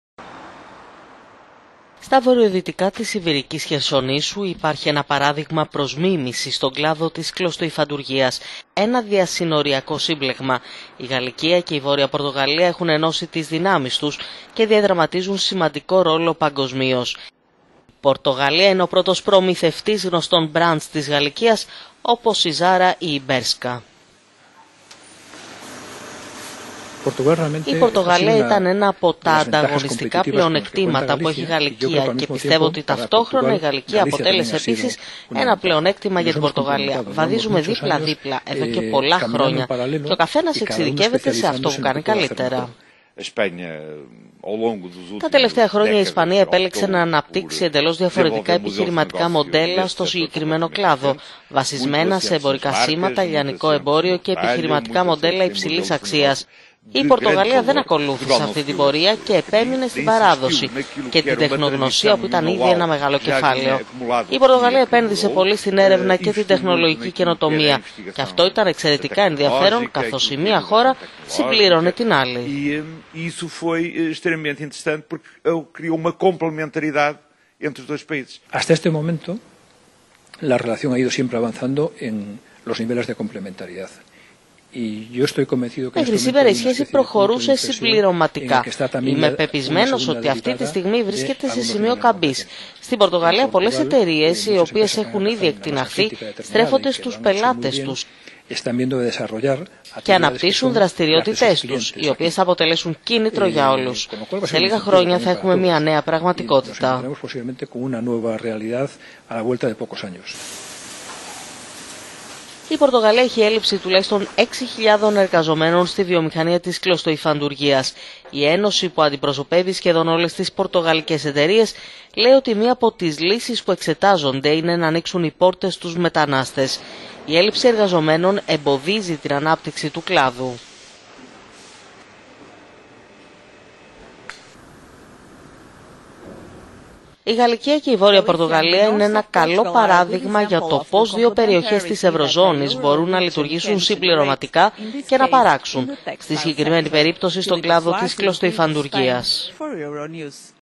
Ενώνουν τις δυνάμεις τους στην κλωστοϋφαντουργία
Η Γαλικία και η Βόρεια Πορτογαλία …